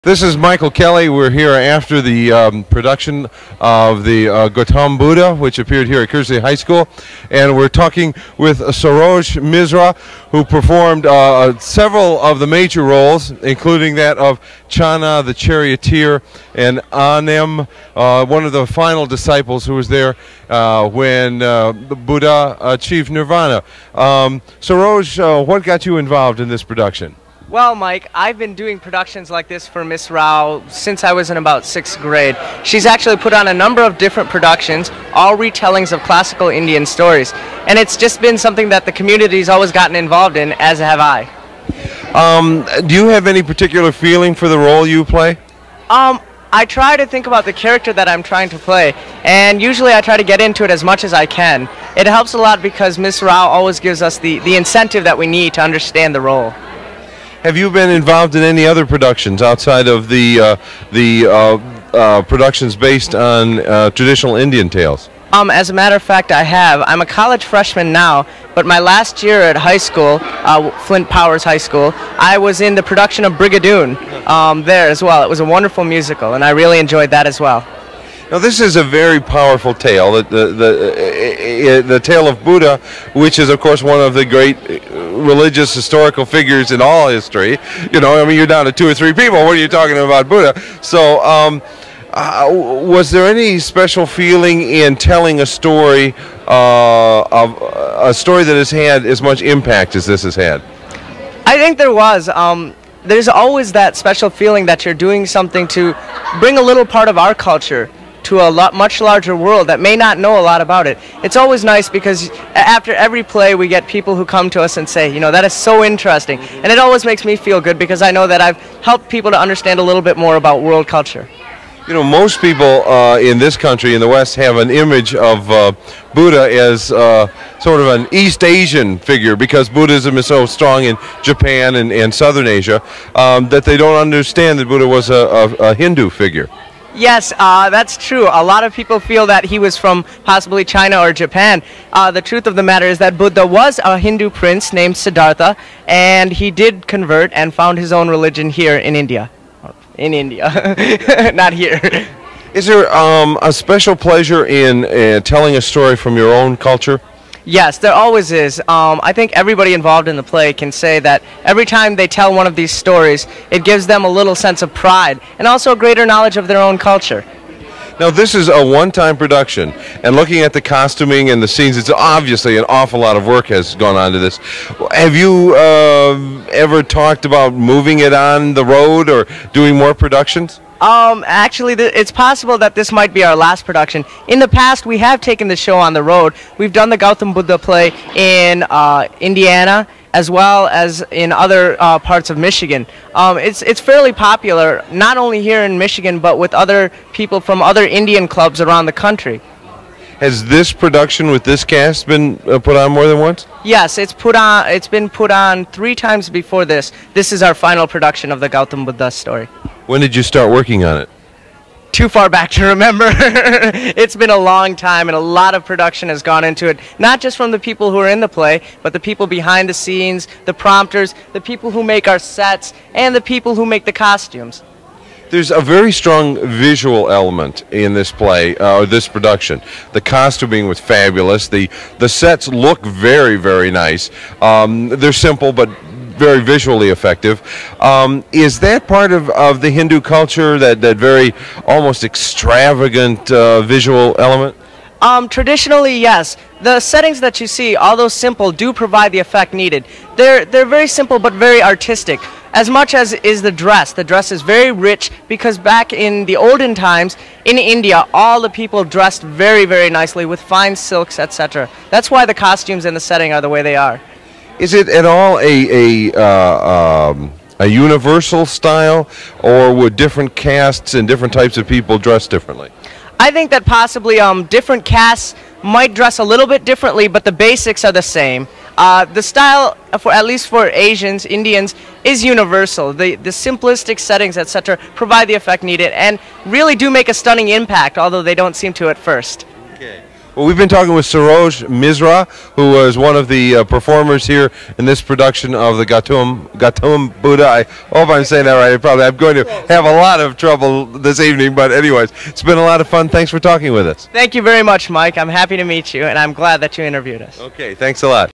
Kelly interviews Saroj Misra cast member of Gautam Buddha
Peerformance of Gautam Buddham: the story of Siddhartha Gautama, Prince of Kapilavastu, plus interviews with cast. Written, choreographed, and directed by Ashoka Rao a leading actress in Hindi community of Greater Flint.

Kelly introduces the production that takes place at Kearsely High School in 1990 (this is a videotape of the production in it's entirety).

Kelly then interviews cast members Saroj Misra, Sachit Malde, Manish Raiji, Aashish Shah, Sruti Puri, Anjali Kartha and Ashoka Rao, writer, director, choreographer, and set designer for Gautam Buddha.

Production: Director - Drew Hahn/John Kotarski; camera - Ken Hannon, Valerie Clarke, Kip Chatterson;